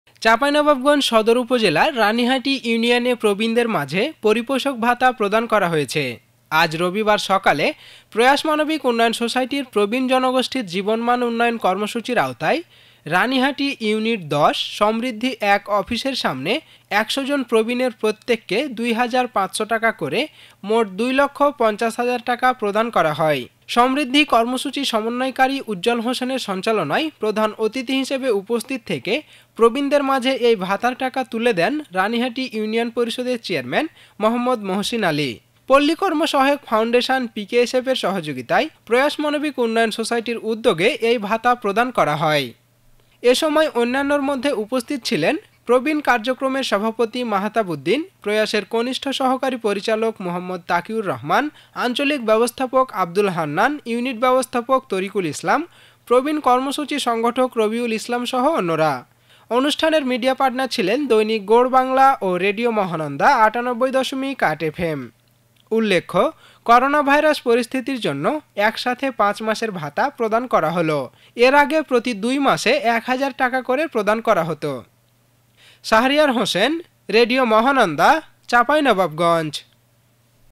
রানীহাটি ইউনিয়নে প্রয়াসের পরিপোষক ভাতা প্রদান
চাঁপাইনবাবগঞ্জে সদর উপজেলার রানীহাটি ইউনিয়নে প্রবীণদের মাঝে পরিপোষক (বয়স্ক ভাতা) ভাতা প্রদান করা হয়েছে। রবিবার সকালে প্রয়াস মানবিক উন্নয়ন সোসাইটির প্রবীণ জনগোষ্ঠীর জীবনমান উন্নয়ন কর্মসূচির আওতায় রানীহাটি ইউনিট-১০, সমৃদ্ধি-১ অফিসের সামনে ১০০ জন প্রবীণের প্রত্যেক ২,৫০০ টাকা করে মোট ২ লক্ষ ৫০ হাজার টাকা প্রদান করা হয়। সমৃদ্ধি কর্মসৃচি সমন্বয়কারী উজ্জল হোসেনের সঞ্চালনায় প্রধান অতিথি হিসেবে উপস্থিত থেকে প্রবীণদের মাঝে এই ভাতার টাকা তুলে দেন রানীহাটি ইউনিয়ন পরিষদের চেয়ারম্যান মো. মহসীন আলী। পল্লী কর্ম-সহায়ক ফাউন্ডেশন (পিকেএসএফের)’র সহযোগিতায় প্রয়াস মানবিক উন্নয়ন সোসাইটির উদ্যোগে এই ভাতা প্রদান করা হয়।